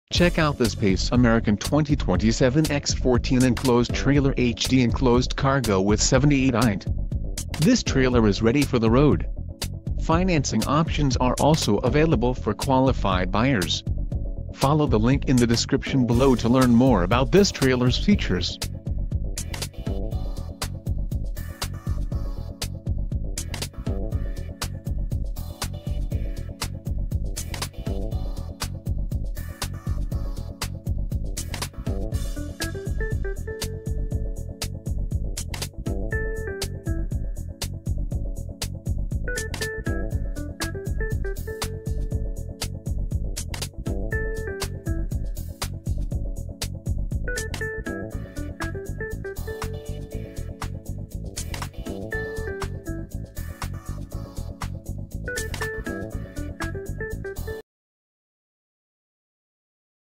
2020 Pace American 7X14 Enclosed Trailer HD Enclosed car...
New Pace JV7X14TE2SE 7X14 and apos; Enclosed Cargo Trailer with 6 Additional Height, V-nose, side door, rear ramp door.  7000 LB GVWR 78 Interior Height 24 V-Nose (2) 3500 LB Axles with EZ Lube Hubs Brakes on all 4 wheels ST205/ 75R15 Radial Tires 16 On Center floor, walls and roof 2 5/16 Coupler 3/4 Water proof floor ATP Rockguard 1 Piece aluminum roof Ramp door with spring assist 32 RV Style side door (2) Side wall vents LED Lights This trailer weighs 2100 LBS Empty ** Please call or email us to verify that this trailer is still for sale** *NO DOC FEES !!! NO INBOUND FREIGHT FEES !!!